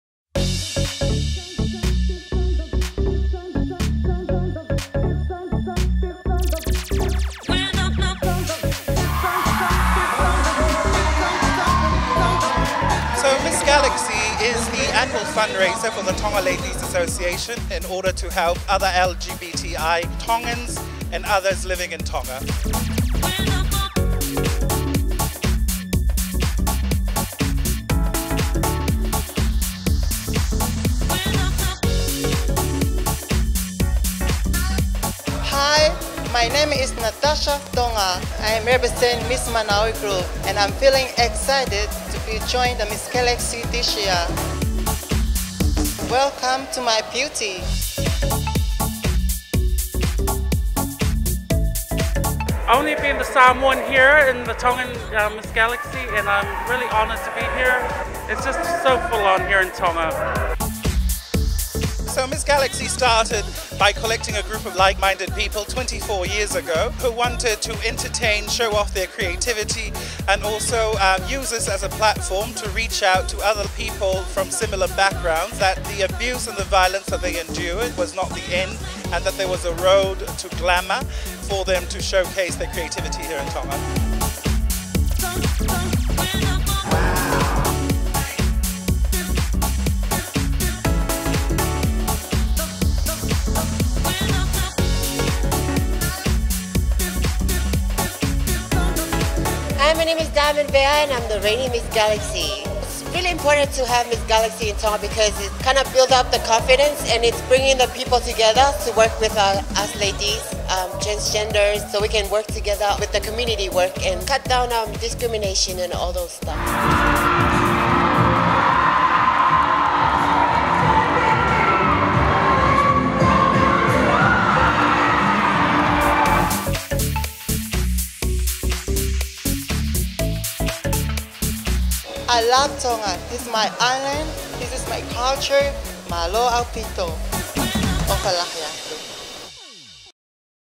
MISS GALAXY - Opening Night Highlights
The 24th Miss Galaxy pageant was held at the Queen Salote Memorial Hall in the Kingdom showcasing the creativity and talents of the leitis.

It was once again an absolutely fabulous show - here are highlights from the opening night.